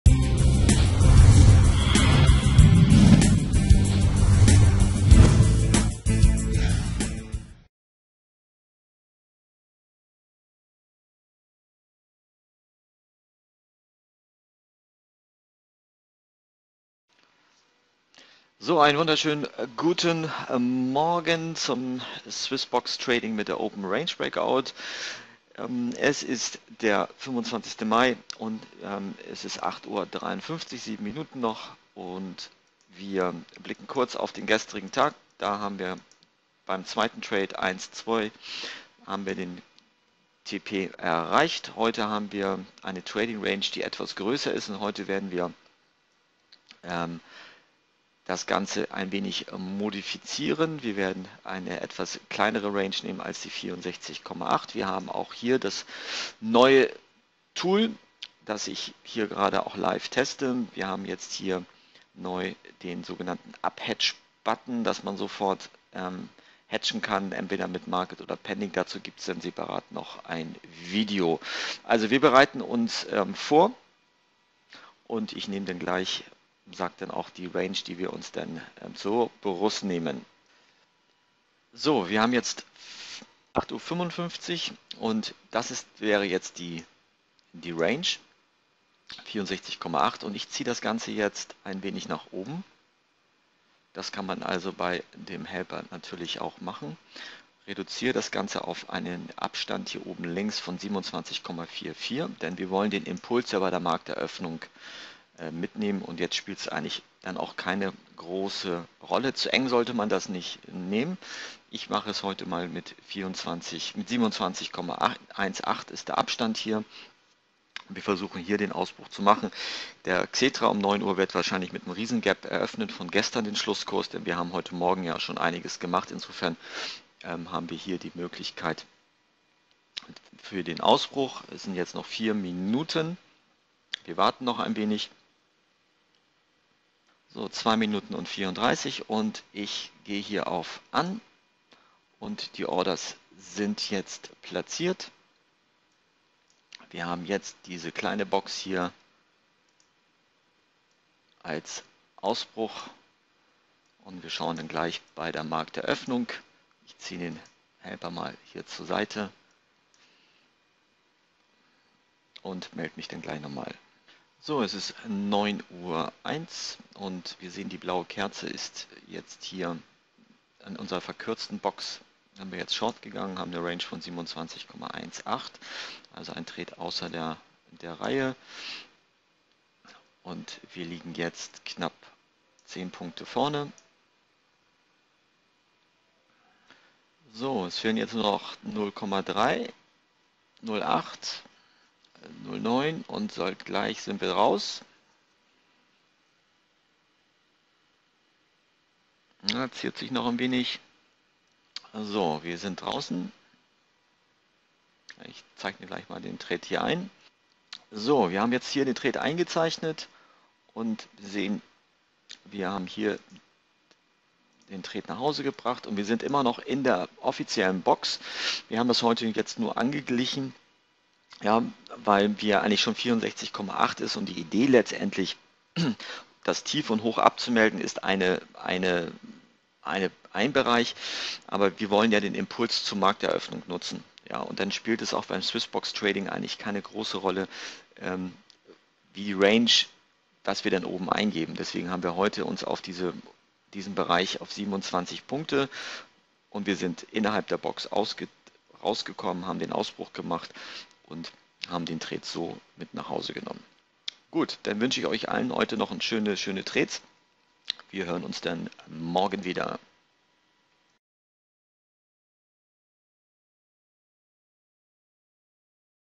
TC24 SwissBox-Trading OpenRange-Breakout Live-Trade am 25.05.2016
Carlos Martins vom TradersClub24 zeigt die Anwendung des SwissBox-Trading mit einem OpenRange Breakout Live-Trade am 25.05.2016. Eine Demoversion können Sie herunterladen.